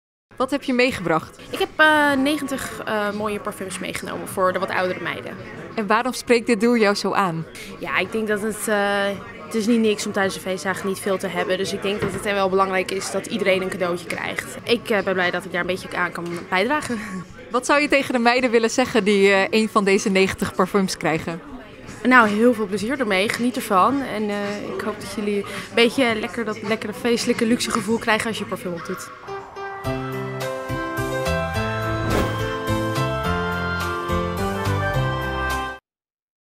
Mascha van Beautygloss kwam langs met 90 parfums
Mascha van Beautygloss.nl bedankt! 😍 Zij doneerde zojuist 90 flesjes parfum. 🙏